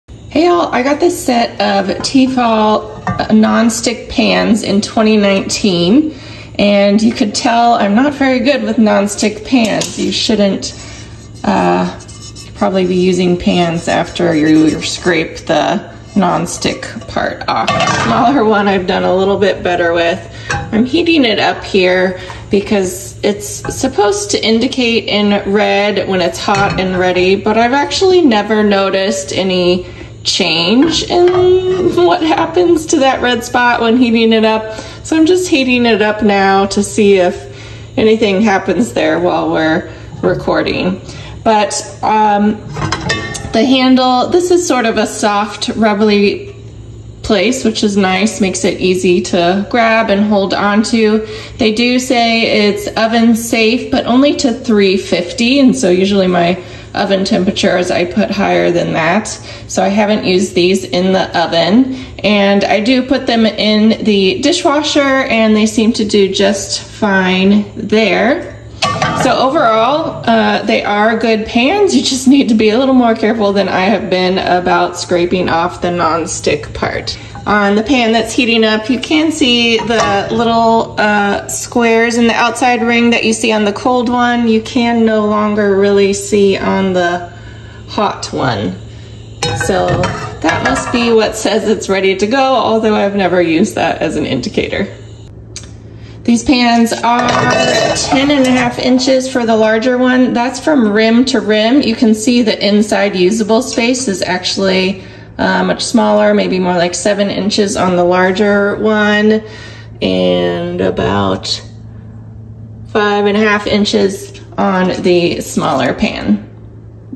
T-fal B039S264 Excite ProGlide Nonstick Thermo-Spot Heat Indicator Dishwasher Oven Safe
Review of T-fal B039S264 Excite ProGlide Nonstick Thermo-Spot Heat Indicator Dishwasher Oven Safe 8 Inch and 10.5 Inch Fry Pan Cookware Set, 2-Piece, Rio Red